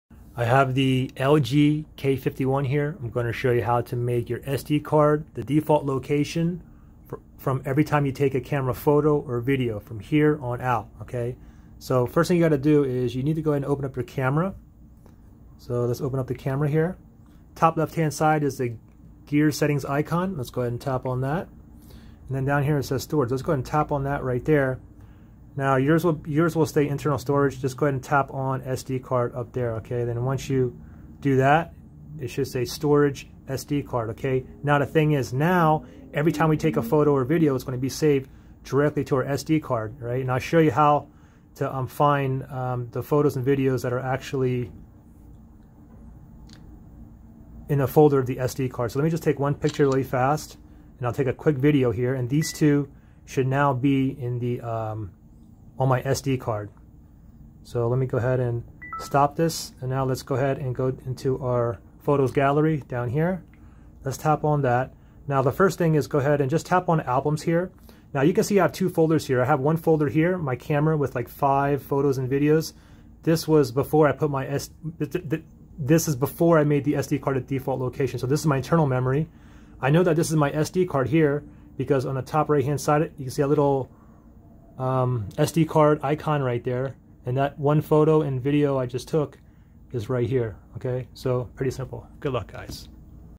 LG K51: How to Make SD Card Default Location for Camera Photos & Videos
I show you how to make the SD Card the default location for camera photos and videos on the LG K51. Once you enable this then thereafter every camera photo or video you shoot will be saved automatically to the sd card. Note the max SD Card size is 2TB. Hope this helps.